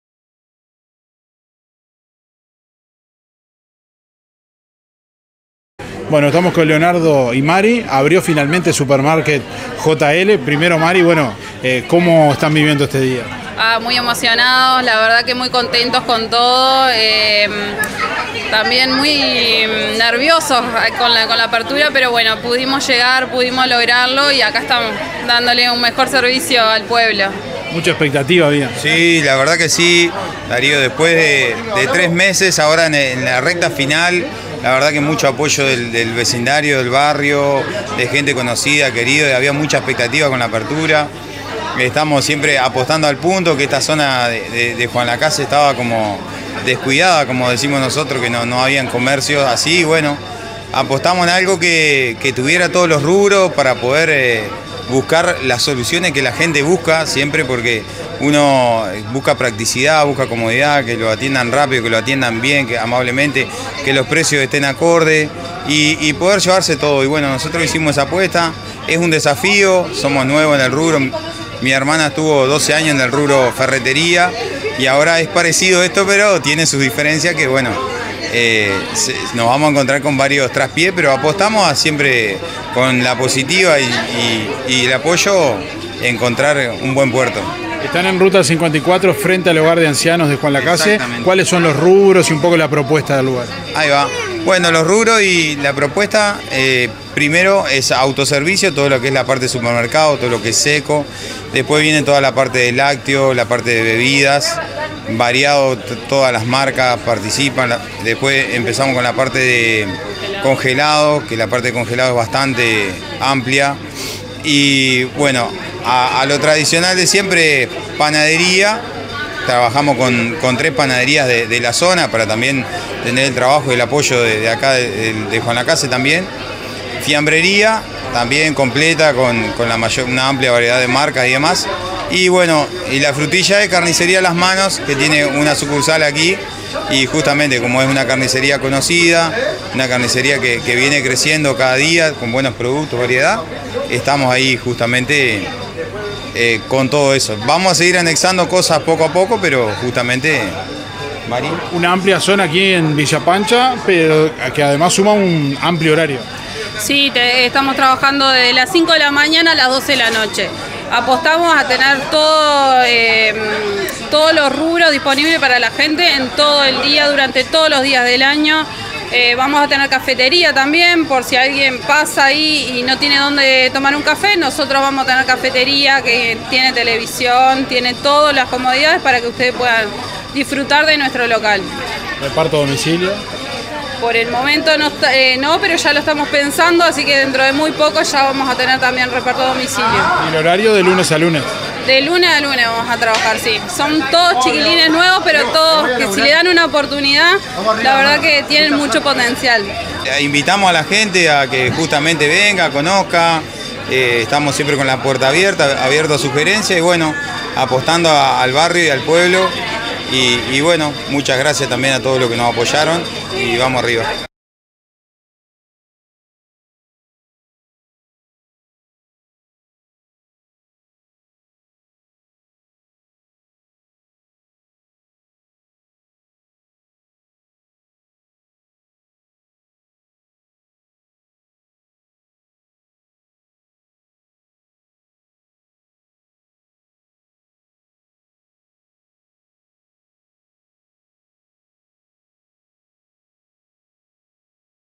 Abrió "El Market JL" en Ruta 54 frente al hogar
Este lunes 29 de agosto a las 17:00 hrs abrió un nuevo autoservicio en juan lacaze: El Market JL en Ruta 54 frente al Hogar de Ancianos.

Con todos los rubros: fiambrería, panadería, verdulería, amplio sector de fríos, congelados, todos los productos de supermercado y sector de Carnicería las Manos.

Además cafeteria y comidas rápidas, con el mas amplio horario corrido de 5 de la mañana a 12 de la noche todos los días.

Hablamos con los hermanos Leonardo y Mary Lima, quienes están al frente de este emprendimiento comercial.